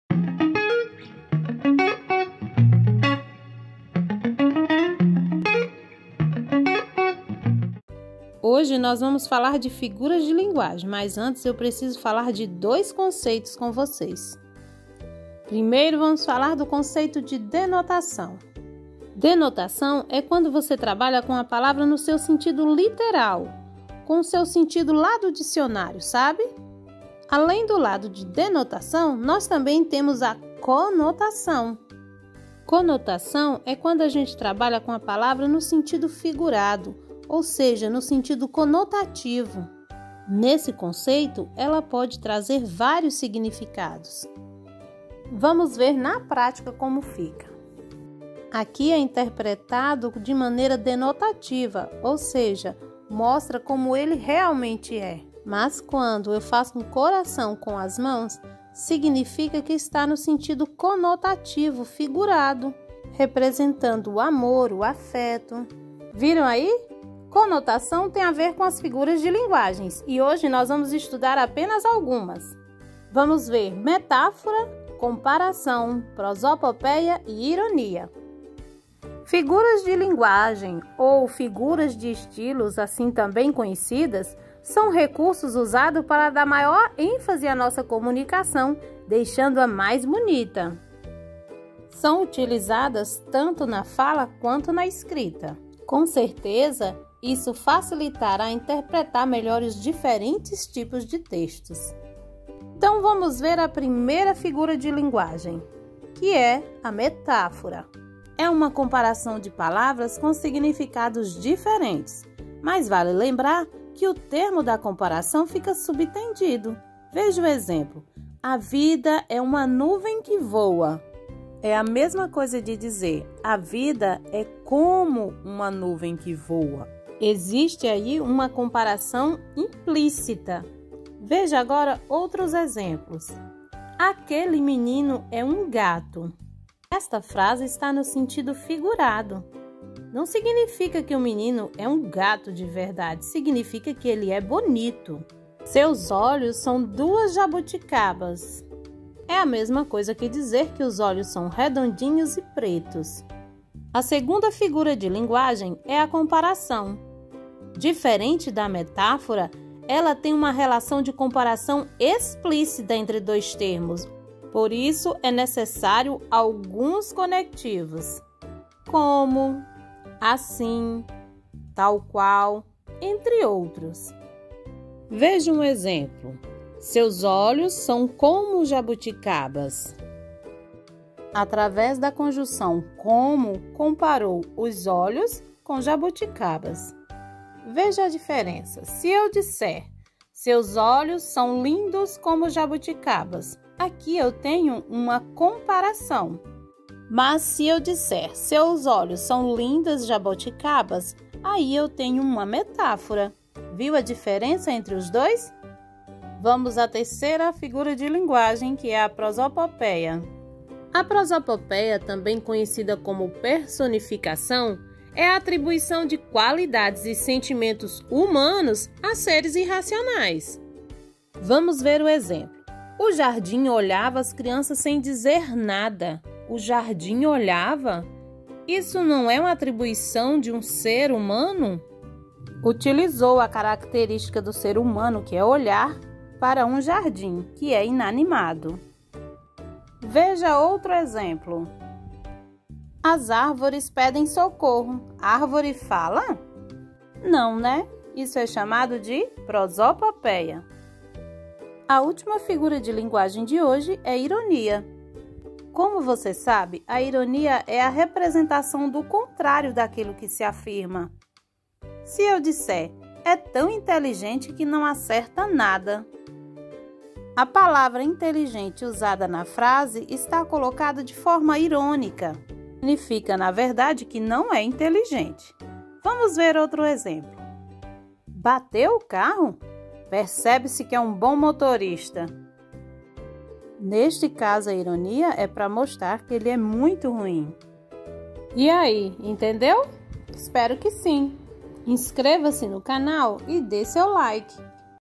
Figuras de Linguagem - Parte 1.
Metáfora, comparação, prosopopeia e ironia.